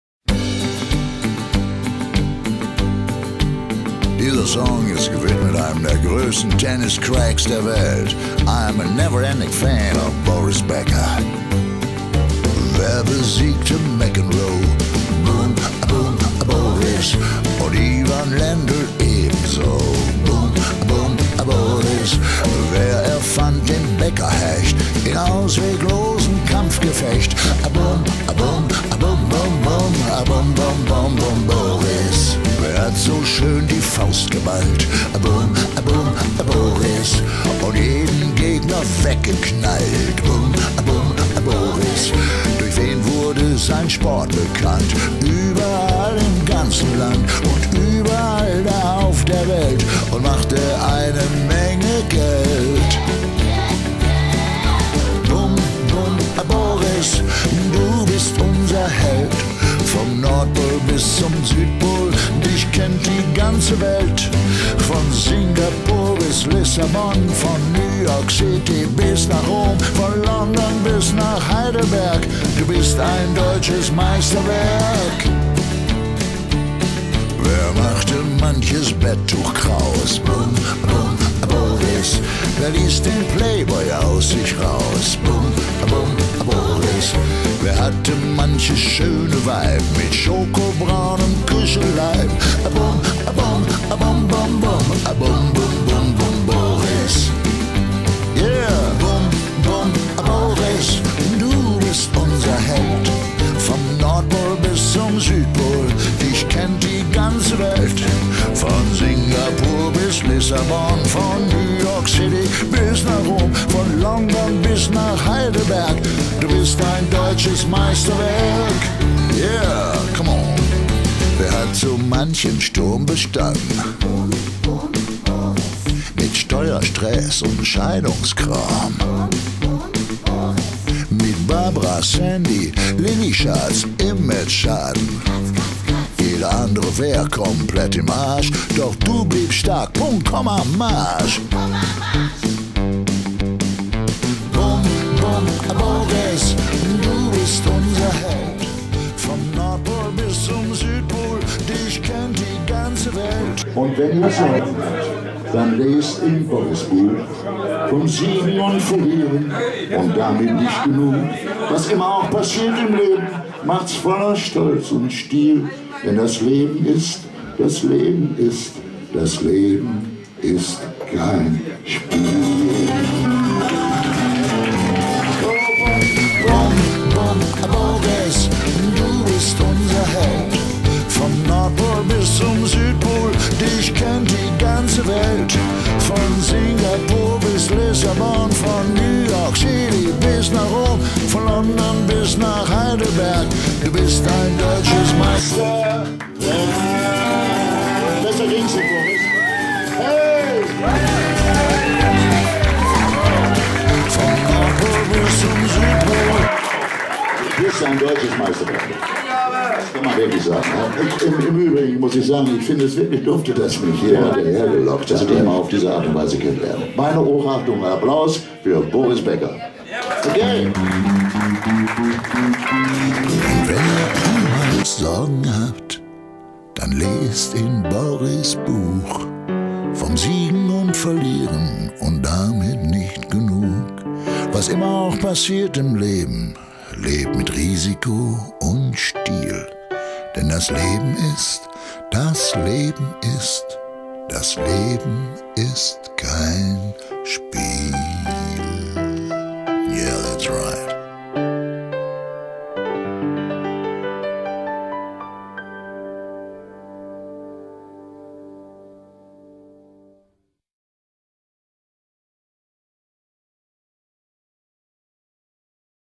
Gunter Gabriel & Boris Becker Geburtstag
deep sadness dear friend. Die Buchparty von Boris Becker.
Christian Schommers, bat Gunter doch zu dieser Buchparty, für Boris einen Song zu schreiben. Wie gesagt - schon getan. An jenem Abend, stand Gunter mit seinen Gitarristen, den neu komponierten Boris Song - frisch gepresst auf CD – wartend vor der Tür des „old commercial room“, um Boris mit seiner komponierten Neuheit zu überraschen. Dieser nichtsahnend  - war zunächst sehr reserviert. Doch der Klang der Gitarren und die ersten Zeilen des Liedes, ließen nach und nach sein Eis schmelzen und zu guter Schluss kam große Freude auf.